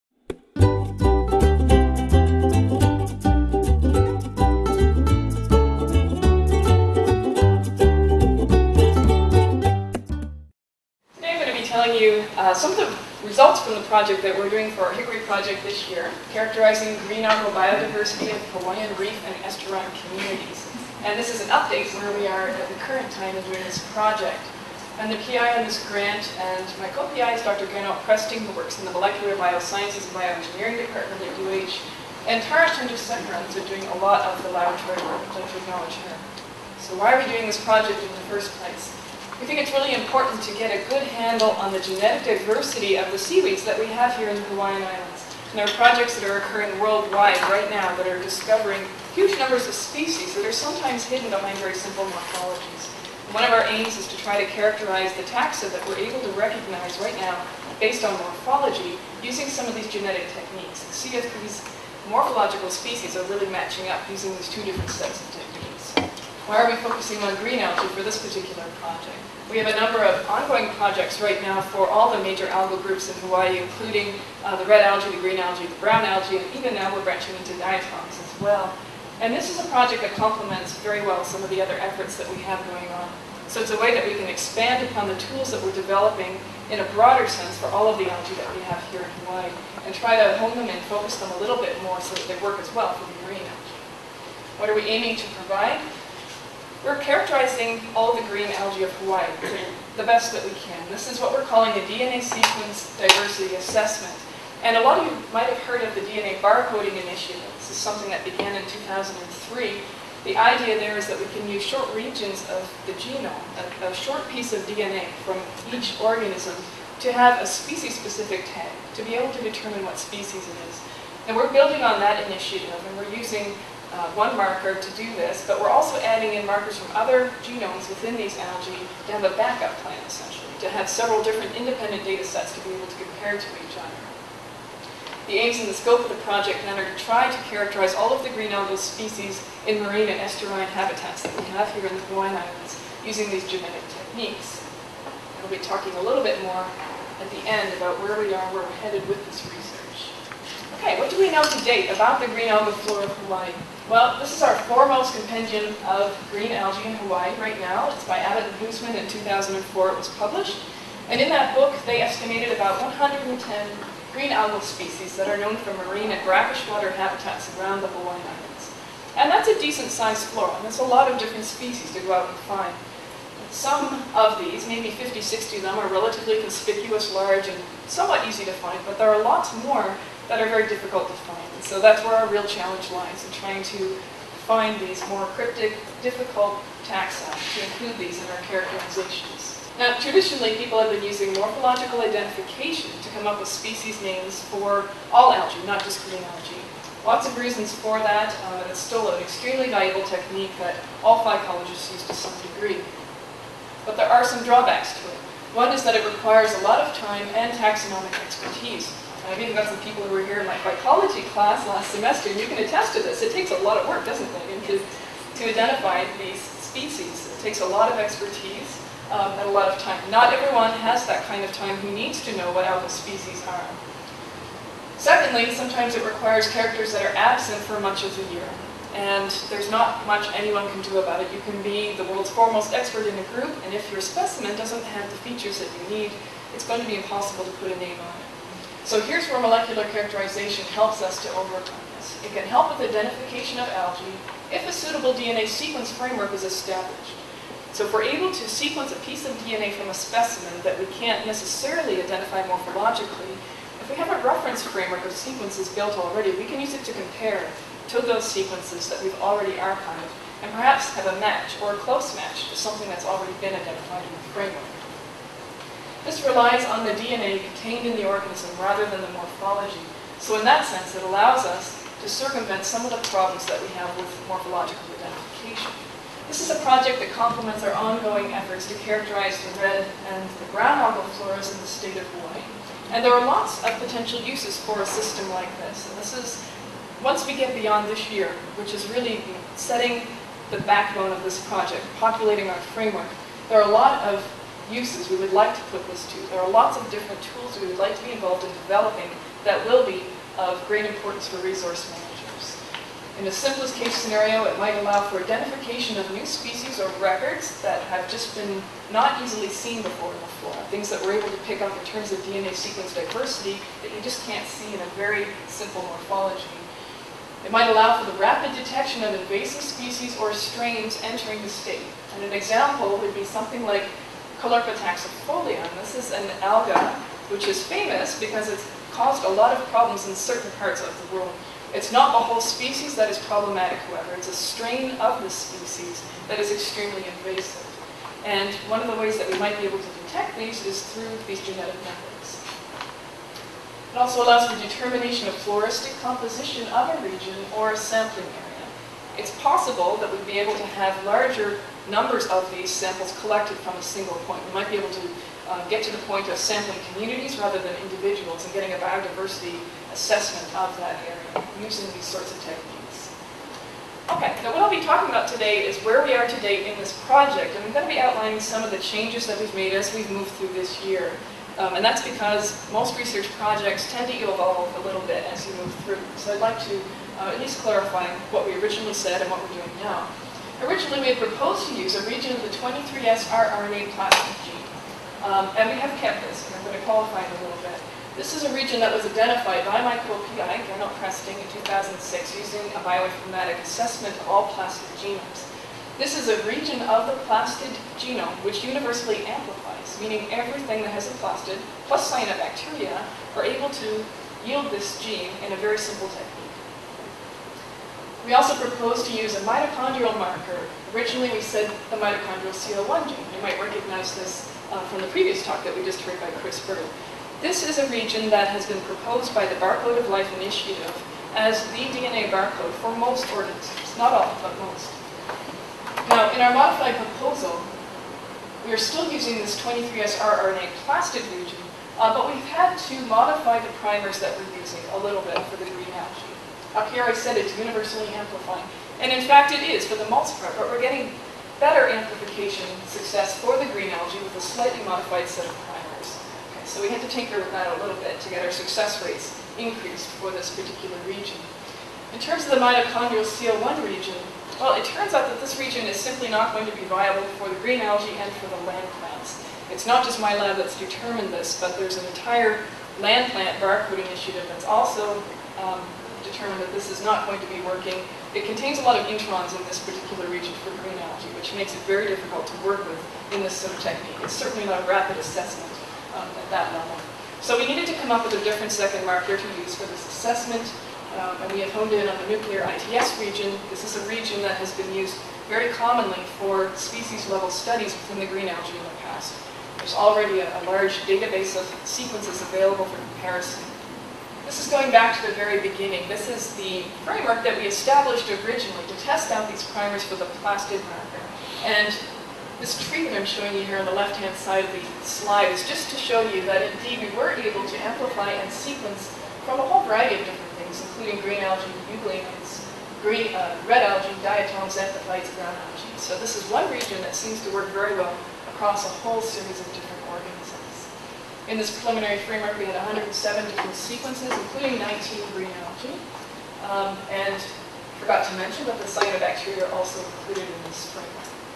- 8/07 - Alison Sherwood - Algal Biodiversity - Pt. 1 -
A presentation on characterizing the green algal biodiversity of Hawaiian reef and estuarine communities, part 1 of 2. HCRI trimester meeting, August 29, 2007.